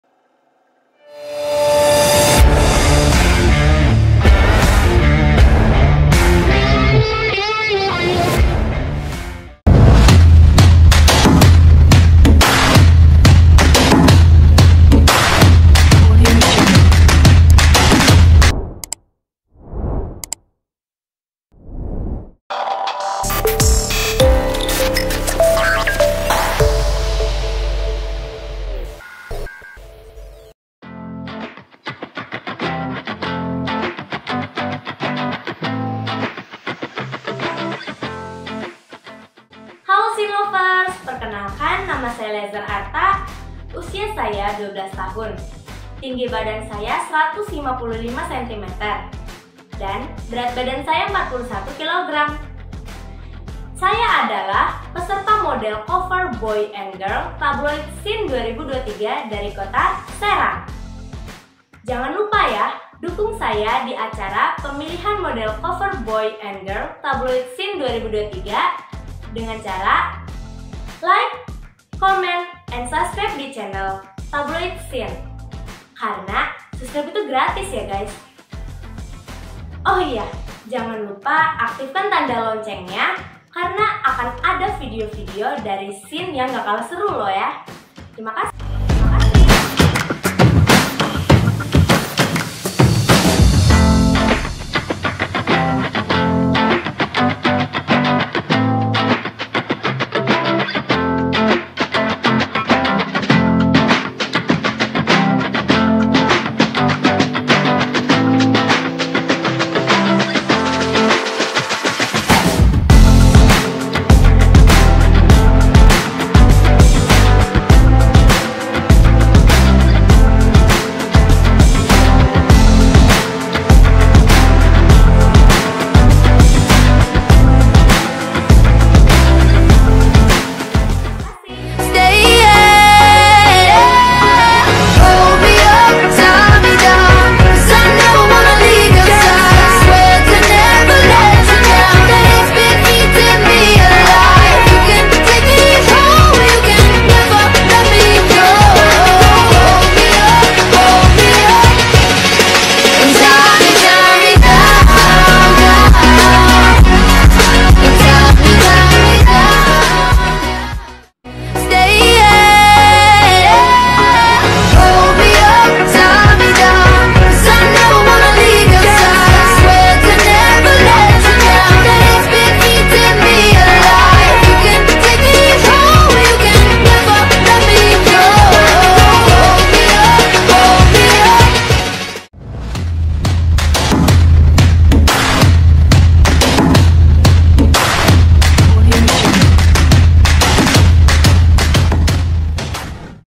ELIEZER ARTHA - 12 TAHUN PESERTA MODEL COVER GIRL TABLOID SCENE 2023 DARI KOTA SERANG BANTEN
Hallo Scene Lover's...
Yuk dukung peserta Favourite kamu di acara  Pemilihan Model Cover Boy - Girl Tabloid Scene 2023 agar menjadi salah satu Pemenang Favourite Youtube...
Makasi...